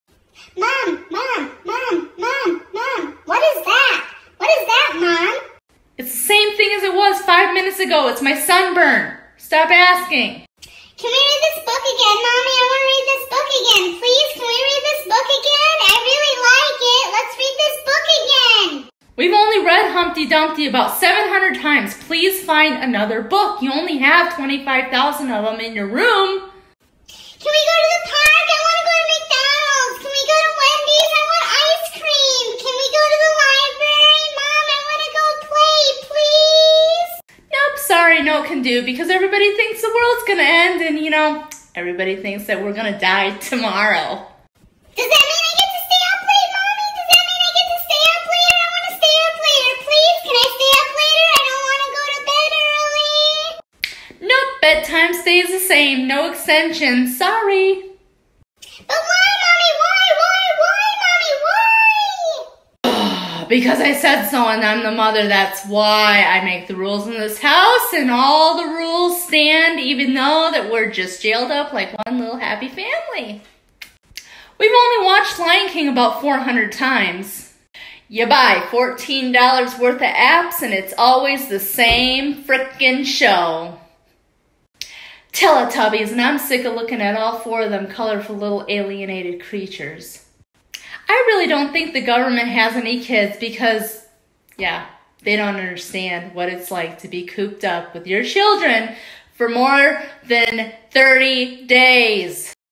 Quarantined with kids
Over a month of fun filled exciting enjoyable times as a family, pretty much jailed up. It’s truly not fun...🥵